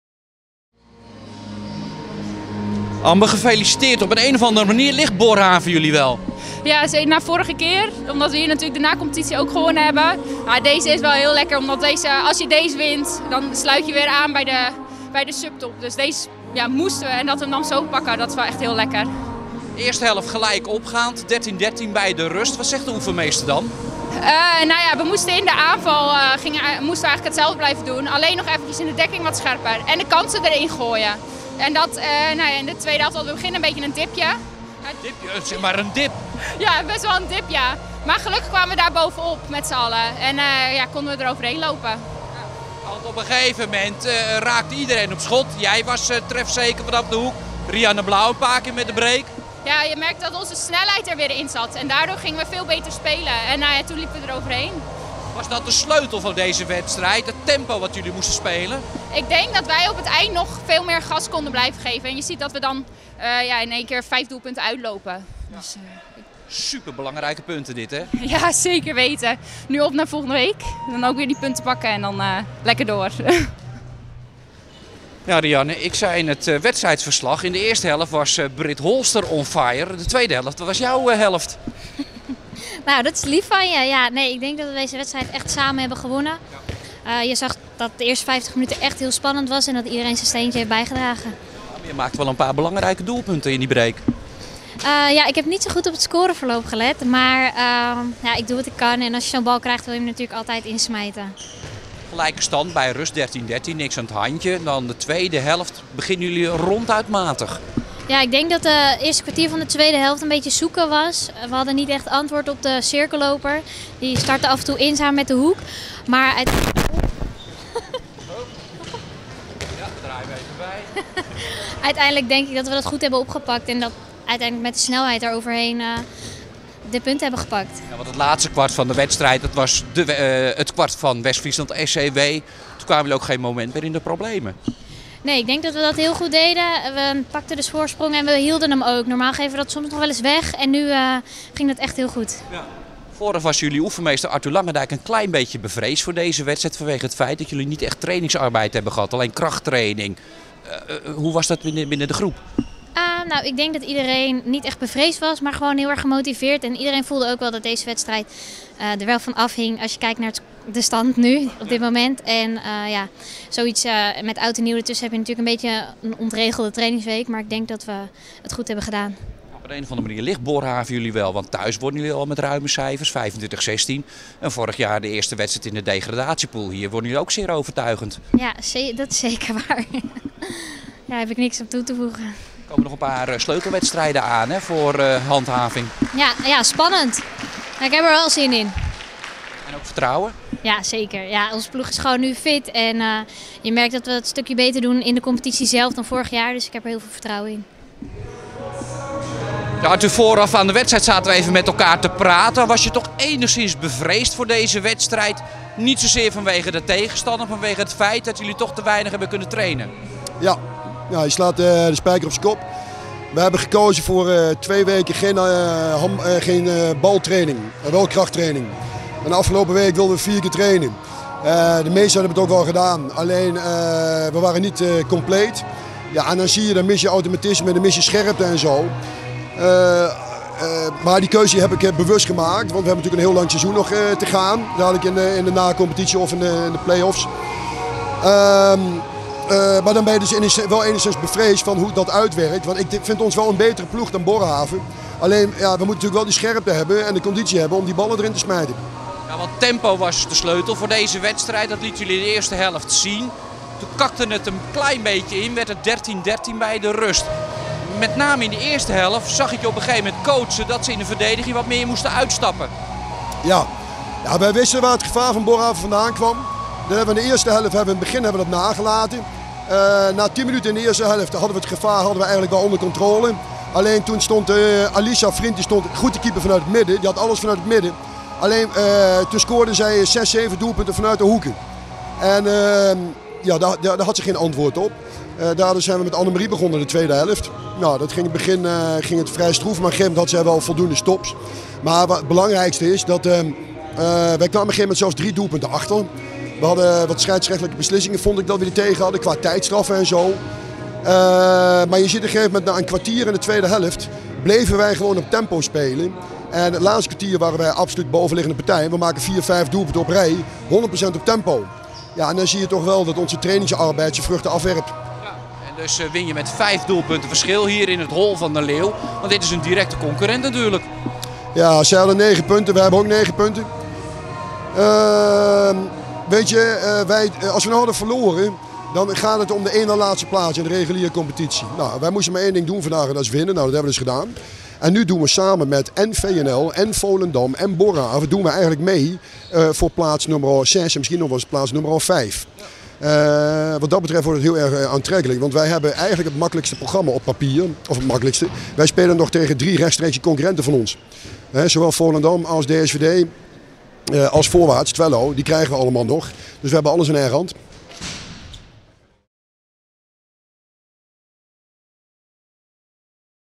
2020-01-05 Zaalhandbal, Eredivisie, Borhave-Westfriesland/SEW, interviews WEEFF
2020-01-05, Zaalhandbal, Eredivisie, Borhave - Westfriesland/SEW, sporthal 't Wooldrik, Borne

SEW zegeviert dankzij fitheid

Borne - De dames van Westfriesland/SEW hebben in Borne het nieuwe jaar opgeluisterd met een klinkende zege op directe concurrent Borhave. In Overijsel liet de ploeg van Arthur Langedijk zien dat, ondanks het korte kerstreces, ze over een prima conditie beschikken en sloegen in het laatste kwart van de wedstrijd toe, 25-29.

Slechts enkele krachttrainingen hadden de ploeg uit Nibbixwoud gehad tijdens de korte winterstop want de gedreven oefenmeester had bewust wat vrijaf gegeven aan zijn ploeg om fysiek en metaal weer fris aan de start te kunnen verschijnen. Het sorteerde effect want direct vanaf het eerste fluitsignaal werd er een hoog tempo ingezet waar de tegenstander uit Borne in mee ging. Het betekende een gelijkopgaande strijd in het eerste bedrijf die met 13-13 werd besloten. Na de pauze was de start van de Westfriese dames ronduit slecht te noemen. Veel foute ballen en knullig verdedigen zorgde ervoor dat er op een gegeven moment zelfs een dubbel ondertal was in het veld wat gelukkig weinig schade opleverde. SEW herpakte zich en ging weer tempo spelen waar Borhave door vermoeidheid steeds meer moeite mee kreeg. Elke slordigheid van de thuisploeg werd genadeloos afgestraft en in dit laatste kwart van de wedstrijd besliste SEW professioneel de goed bezochte partij, 25-29.

Na afloop sprak onze verslaggever met hoekspeelster Amber Luppens en Riejanne Blaauw van Westfriesland/SEW, alsmede hun trainer/coach Arthur Langedijk.